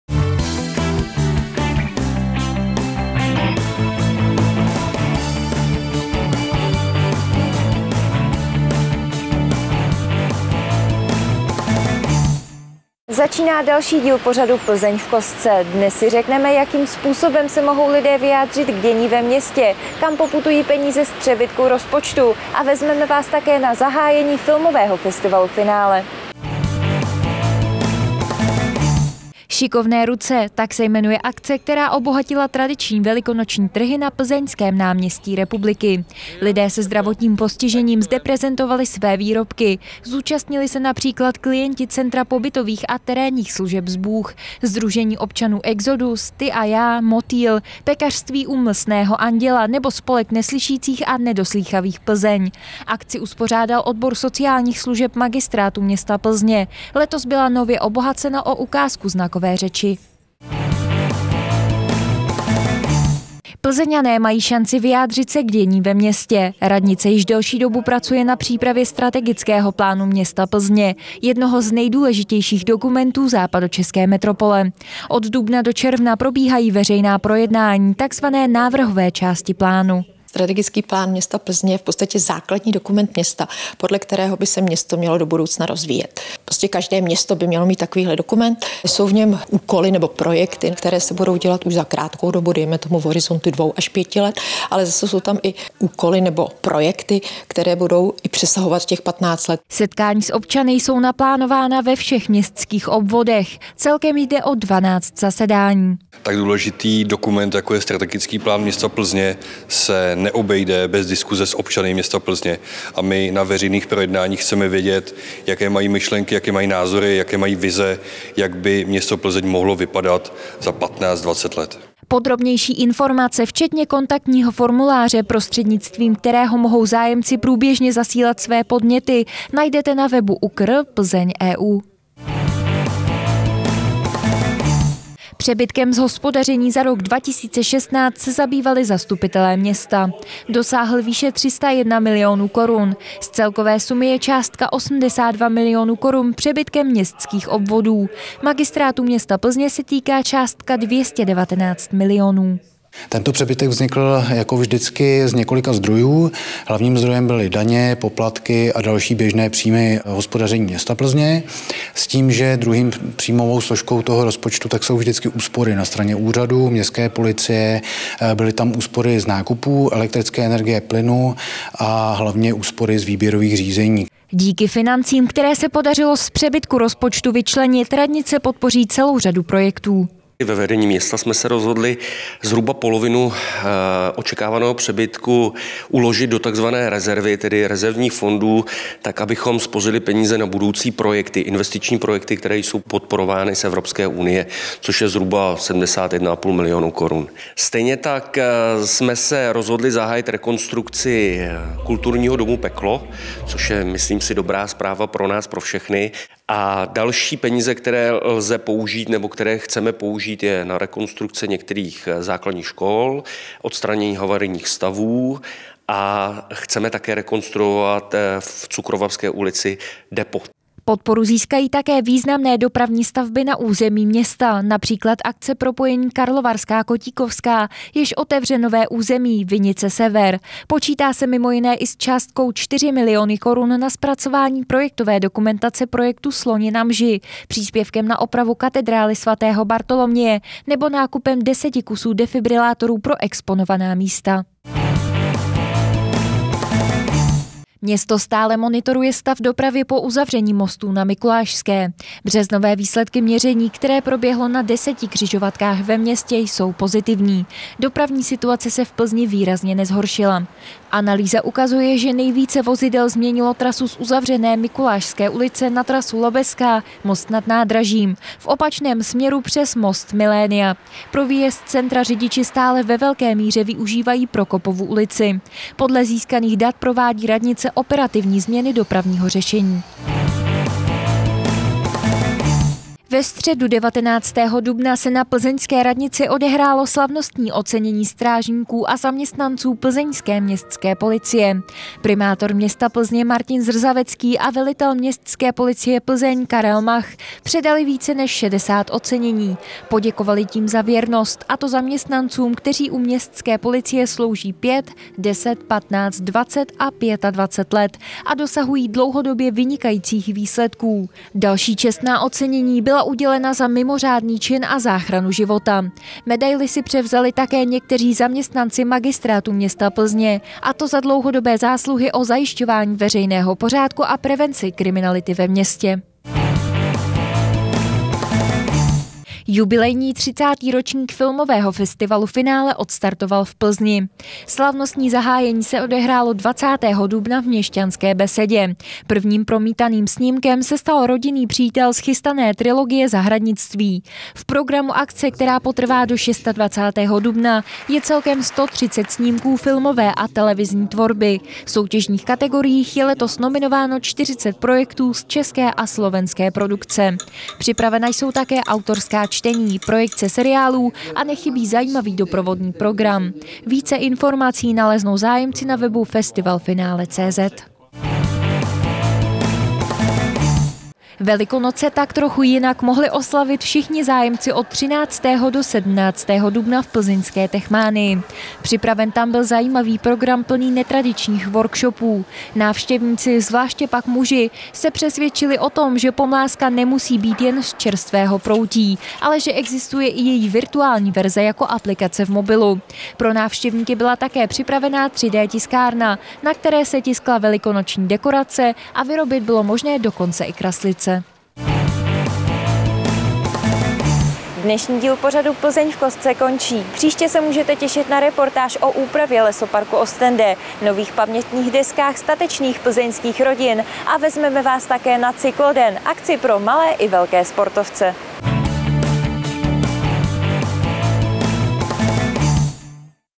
Plzeň v kostce (24.4.-30.4.2017)
Reportáže:
Šikovné ruce se po roce vrátily na náměstí Republiky
Přijďte společně s námi tvořit ještě lepší Plzeň
Peníze z přebytku podpoří řadu akcí
Uzavírka Mikulášské ulice nepůsobí vážné komplikace
Primátor ocenil nejlepší strážníky městské policie
Filmový festival Finále odstartoval, nabídne přes 130 snímků
Děti i dospělí si užili Velikonoce v Techmanii
Město testovalo protipovodňové zábrany